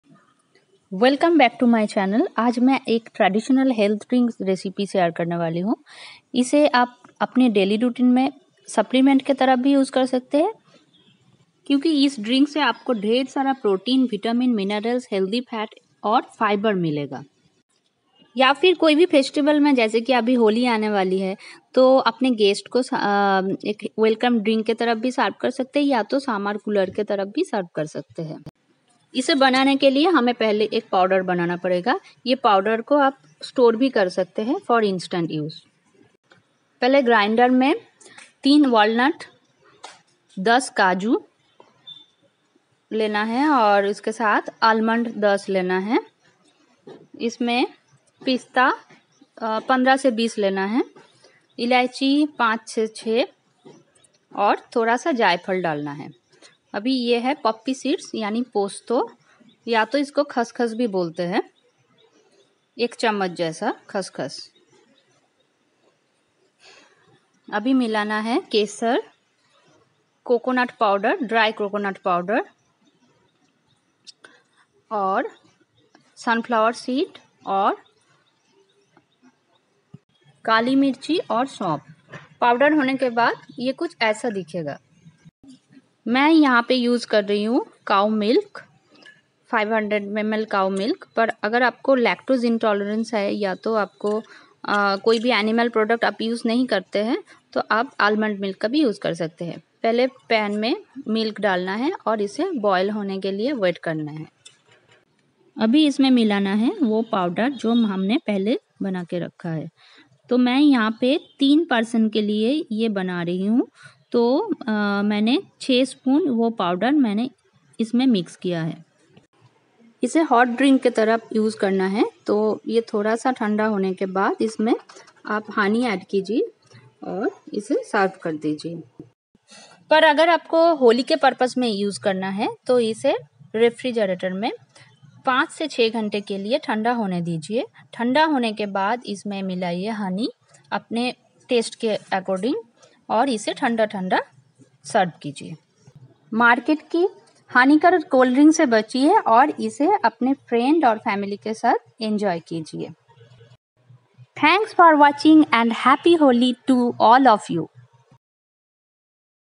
No sugar Thandai /Cooler/Healthdrinks@healthnutritionentertainme6326
Without thandai holi celebration is incomplete.This fresh homemade thandai tastes absolutely heavenly.Thandai is the perfect drink to serve on special days and occasions like Holi,Diwali etc.